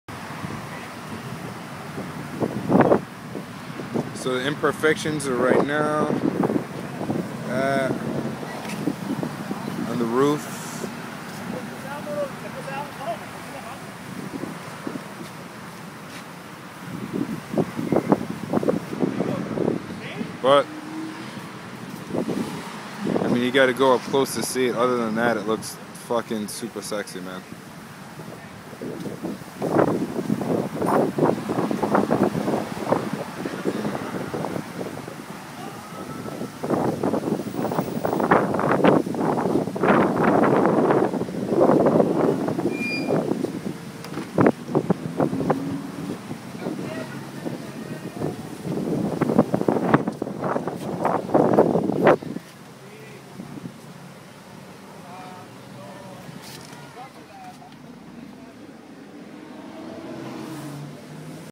95 supra for sale $15,000 obo
great conditions, runs perfectly, i use it daily. body has normal dings from usage nothing major . it was red i painted in black last year.
body has 150k miles. However the engine and tranny have 90k miles.
car was orginallyy automatic was changed to 5 speed.
it is a 95 so i had to do alot... alot of workk to it here are some parts a changed/ replaced
- ac and compressor ( )
- altenator , starter , suspension ( adjustable coilover so you can rainse or drop the car as you please)
- changes both brakes today may 14 2012
- new clutch , custome trunk with 10" sub , dvd player in center console
this car is ready to drive period whether every day or weekend. my orginal plan was to put in turbo but then you realize theres alot of important things you have to do first like ac/  starter altenator ect c above. the body looks amazing but as i said has minor cosmetic chirps scratches, be ware alot of older cars hence.. supras might look great but you will have to do a lot to make it ur everyday car I've done all hard/expensive works drives perfectly no problems .